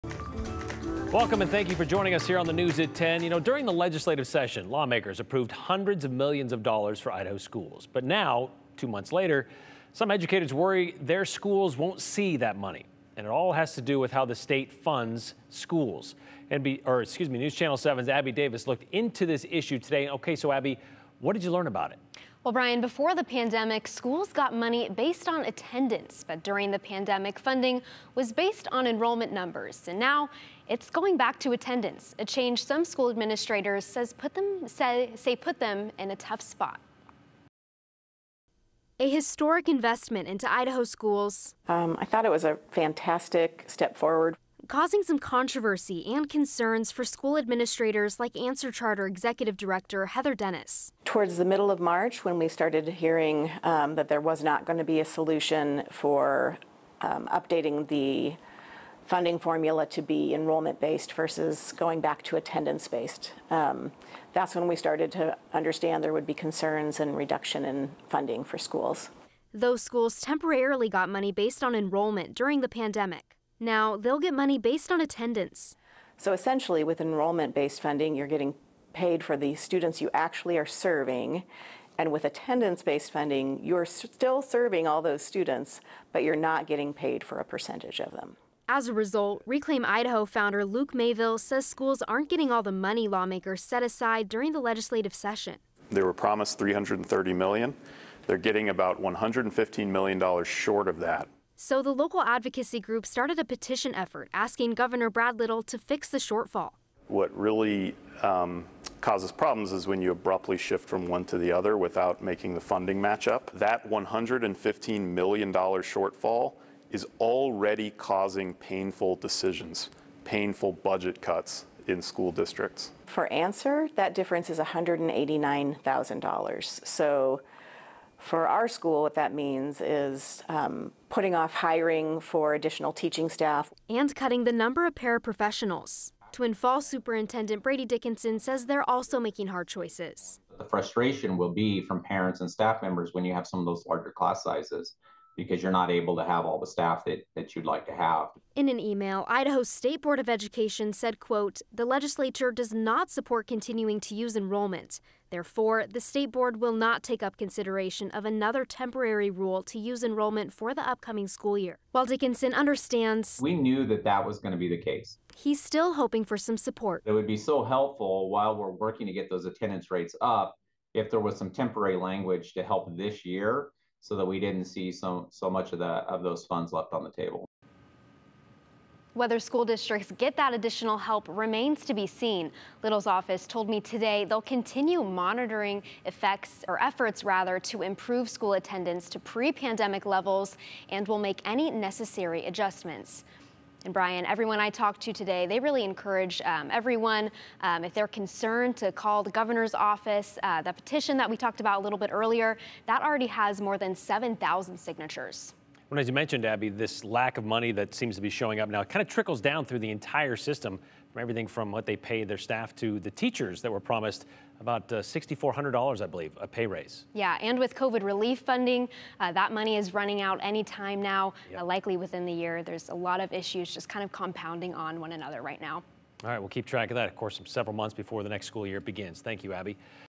K-12 funding concerns in Idaho
Schools are worried they won't have enough funds based off of attendance.